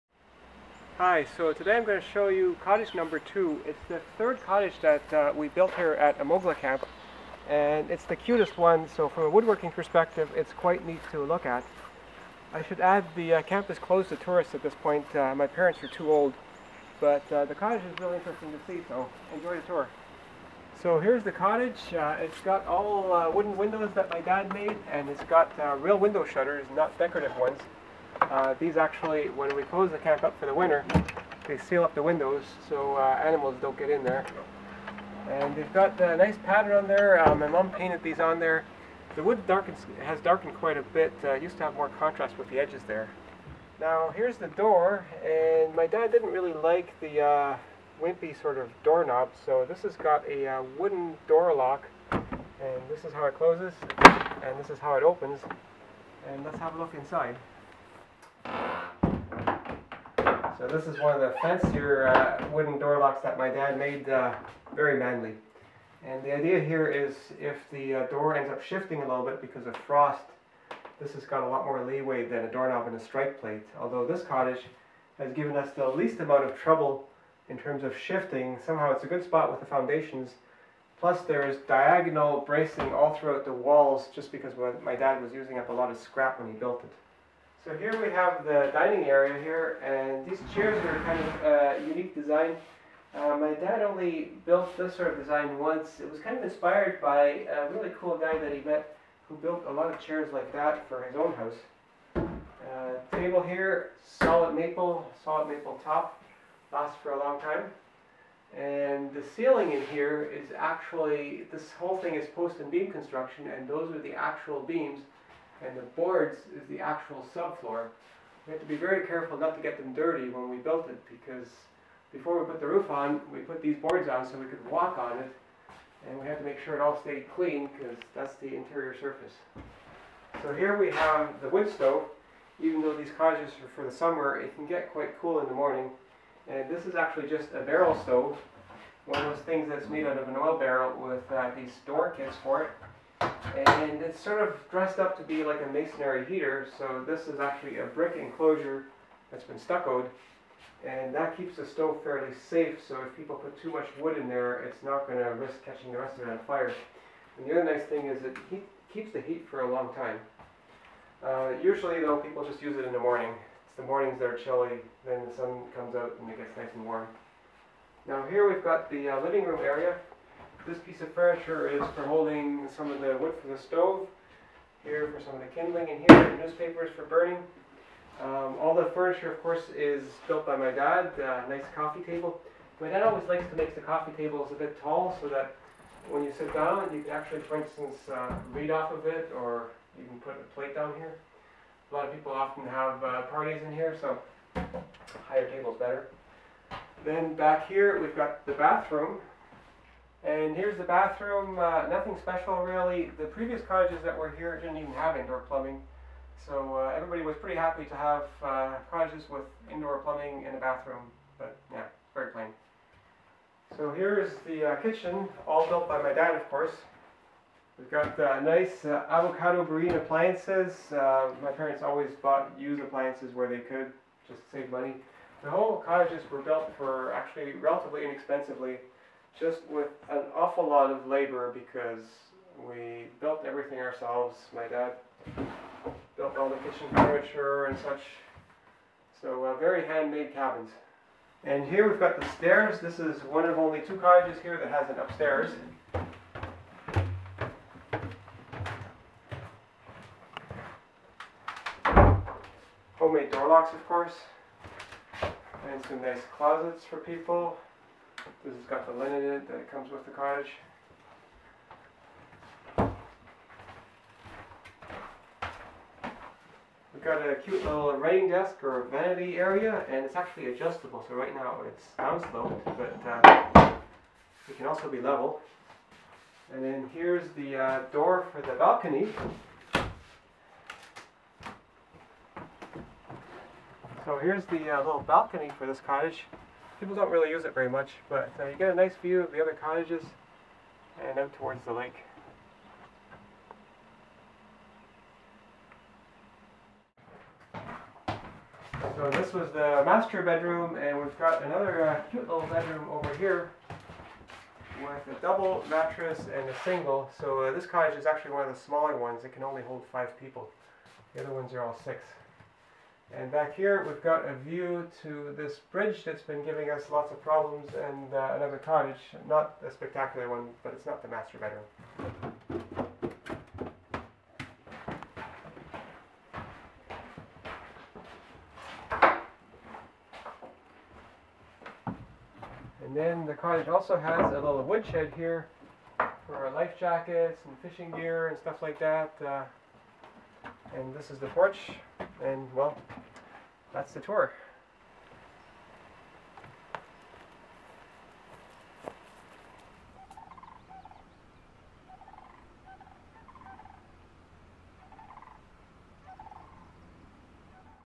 Touring cottage #2 at Amogla camp
Showing off one of the cottages at Amogla Camp.
Amogla camp is no longer open to tourists.